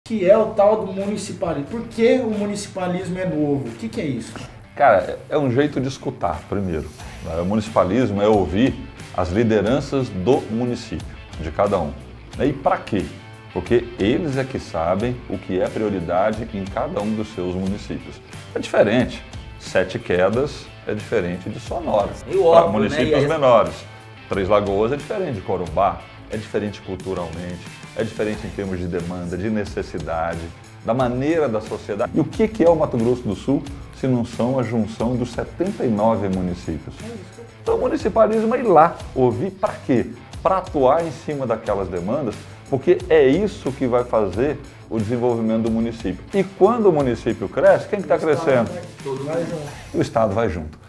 Municipalismo
Municipalismo é um jeito de escutar as prioridades de cada cidade e transformar os anseios em investimentos para as pessoas.

É assim que vamos fazer o desenvolvimento dos municípios acontecer, gerando melhorias nas diversas áreas como infraestrutura, saúde, educação e segurança pública.